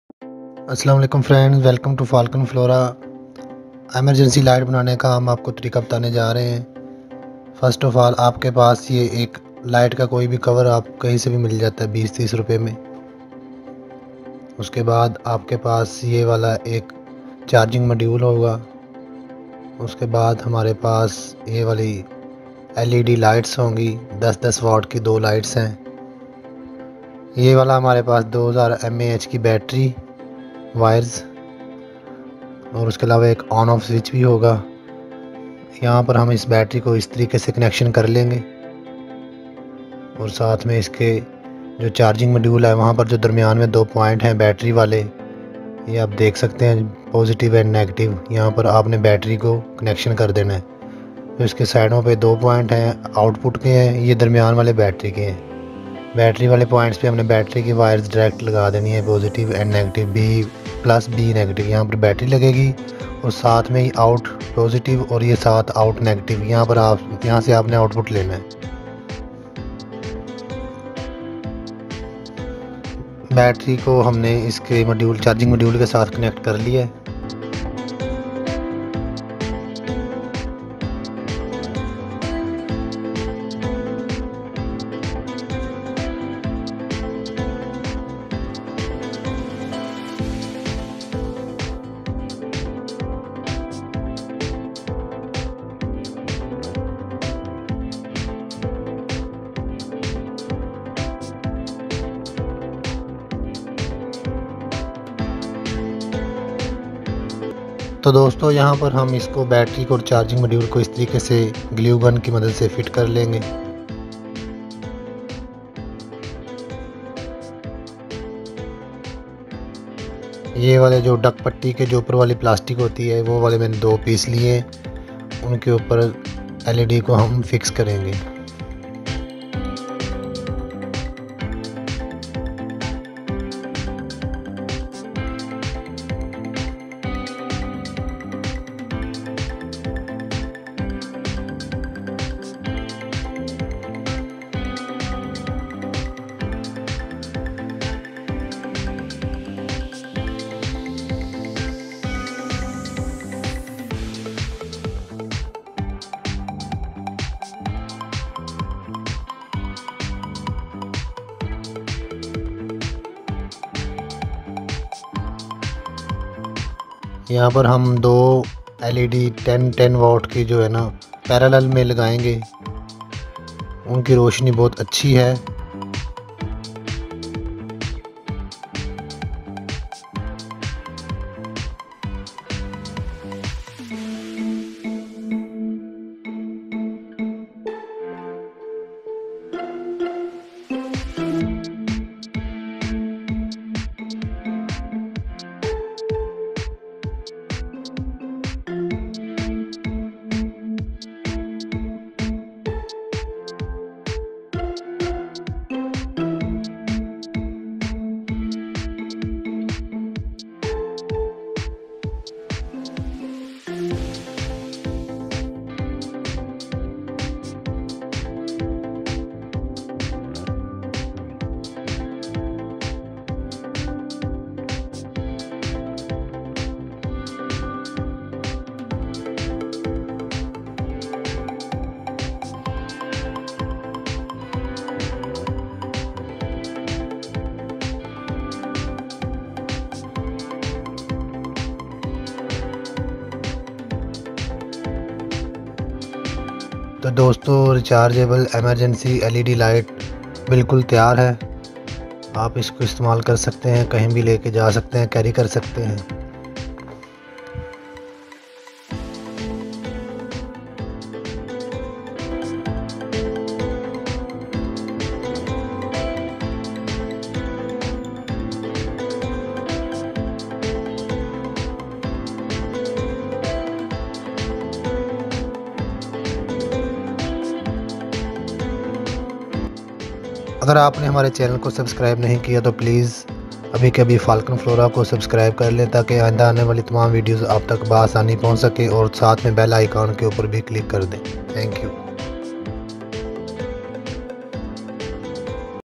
Rechargeable Emergency LED Light 20Watt
Hi Friends!Today i have shared 20watt Rechargeable LED Light..

For more queries please comments here ...

Falcon Flora channel is most useful channel for Electronics,
 Mobile Software+Hardware Solution...